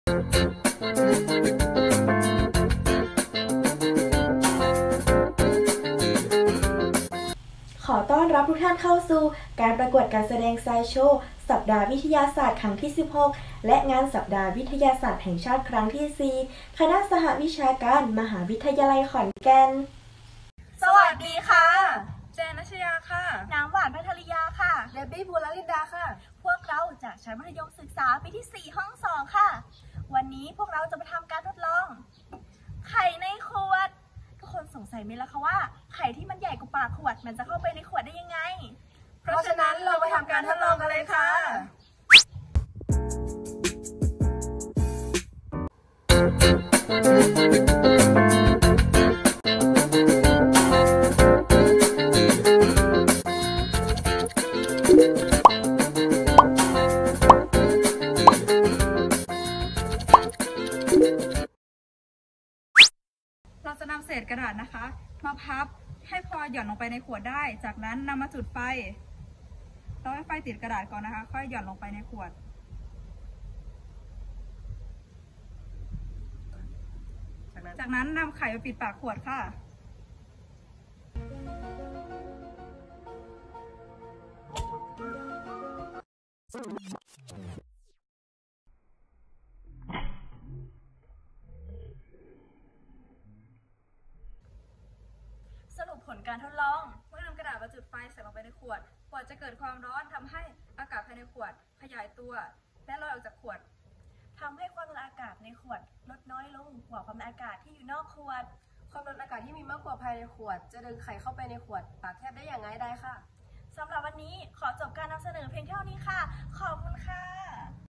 (ม.ปลาย) ดูดไข่เข้าขวด โรงเรียน เพ็ญพิทยาคม
วิดิโอนี้เข้าร่วมกิจกรรม การแสดงทางวิทยาศาสตร์ (Science Show)
ประเภท มัธยมศึกษา ตอนปลาย งานสัปดาห์วิชาการครั้งที่ 16 และสัปดาห์วิทยาศาสตร์แห่งชาติครั้งที่ 4 ปี 2021 คณะสหวิทยาการ มหาวิทยาลัยขอนแก่น

ผู้เข้าร่วม
1. ภูรลินดา เชือกพรม
2. ภัทรยา ผาทอง
3. ณัฐชยา ชนชิด

ครูที่ปรึกษา
คุณ เบญจมาศ กระตารัตน์
คุณ ปรีชา ดีนาง